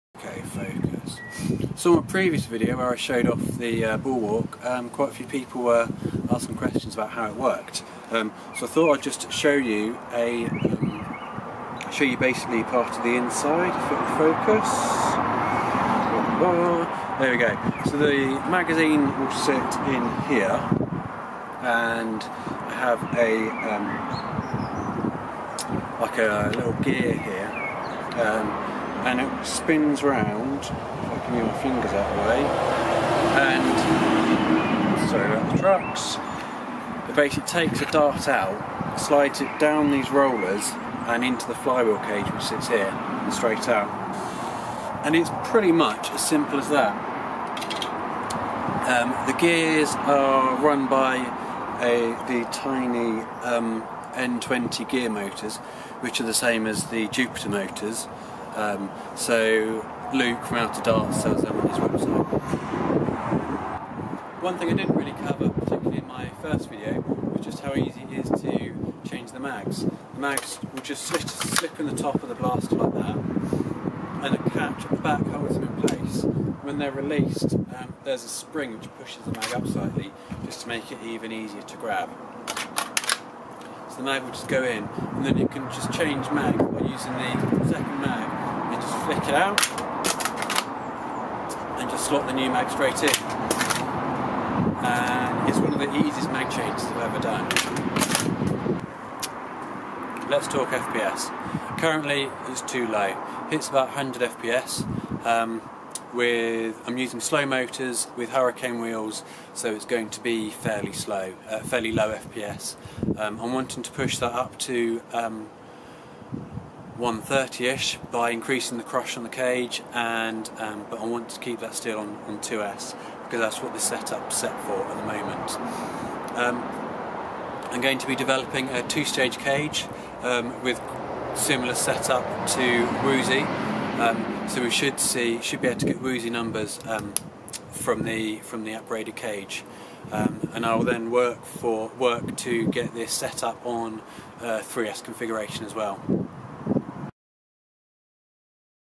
Bulwark, a bit more info
This video goes into a bit more information about how the blaster works, the mag changes and fps